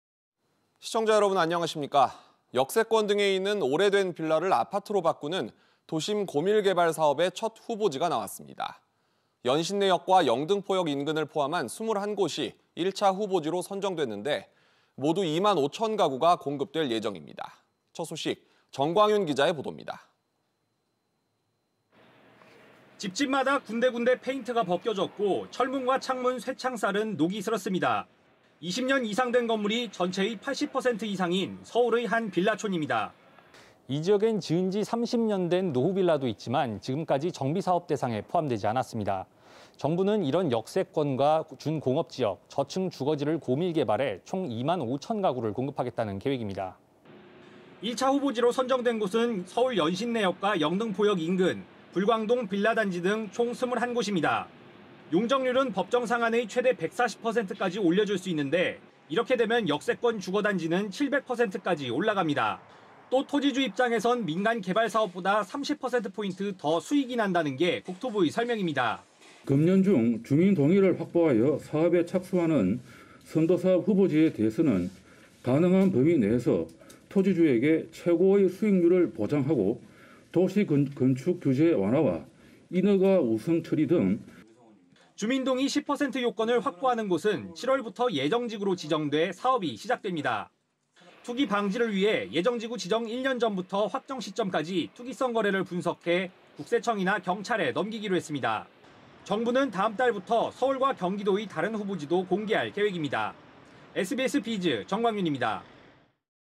노후 빌라촌이 아파트로…연신내역 등 21곳 후보지 선정
[앵커]

역세권 등에 있는 오래된 빌라를 아파트로 바꾸는 '도심 고밀개발 사업'의 첫 후보지가 나왔습니다.

연신내역과 영등포역 인근을 포함한 21곳이 1차 후보지로 선정됐는데, 모두 2만5천 가구가 공급될 예정입니다.

정광윤 기자의 보도입니다.

[기자]

집집마다 군데군데 페인트가 벗겨졌고 철문과 창문 쇠창살은 녹이 슬었습니다.